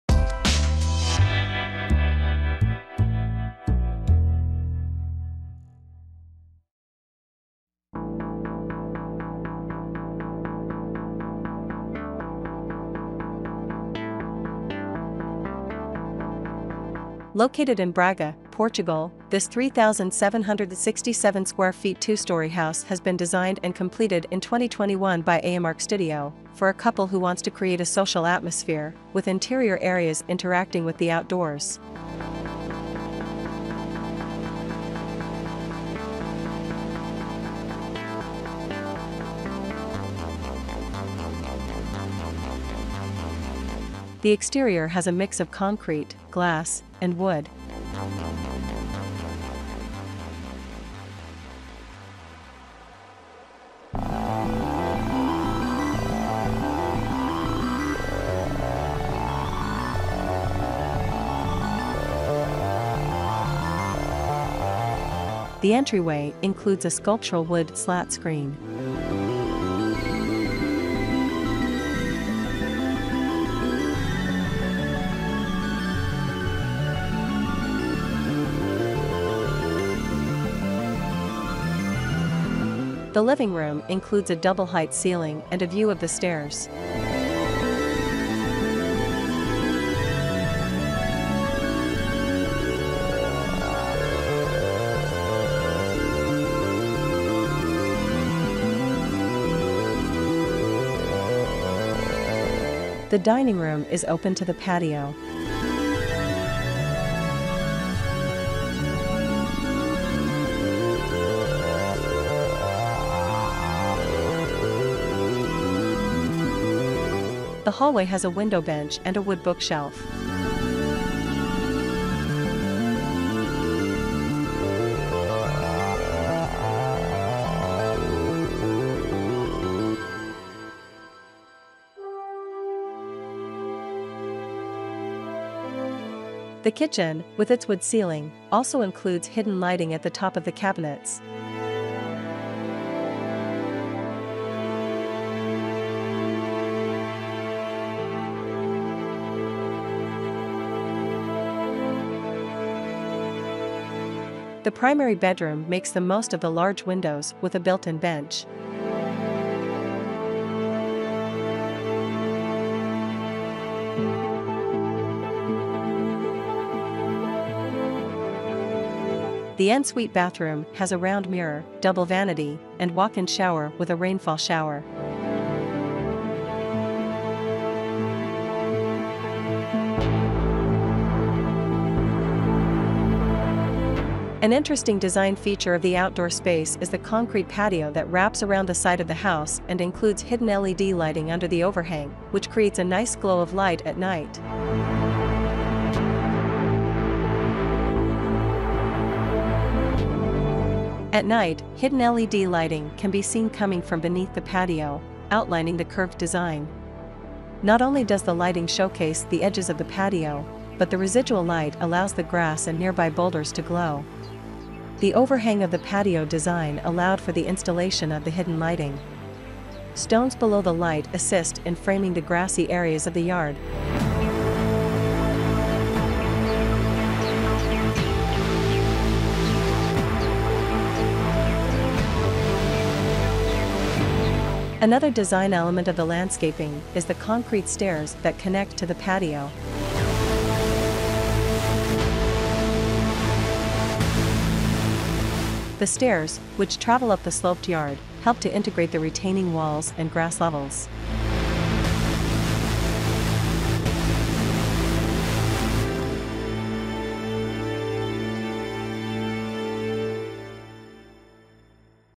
15 House, The Home In Portugal With Landscaping Design Idea
Every project starts with a client, a place, and a program. This one began with a couple, a conversation, and a model made with the objects that were on the table. At that moment, there was the recognition that the shape that the customers had in mind, even without even knowing it, had been reached.

The conversation led to a two-story prototype. The private space models the upper volume, more closed, almost like a shell, and the lower volume creates freedom for the visual field.

The user feels free to walk through the common spaces, which creates a greater spatial fluidity.

All spaces have details, such as the furniture design, that were thought and integrated in the architectural proposal from the beginning.

Engineering : Eduarda Oliveira
Constructor : RespirarArte
Interior Decoration : AM-arqstudio
Location : Braga, Portugal
Area : 3767 ft²
Year : 2021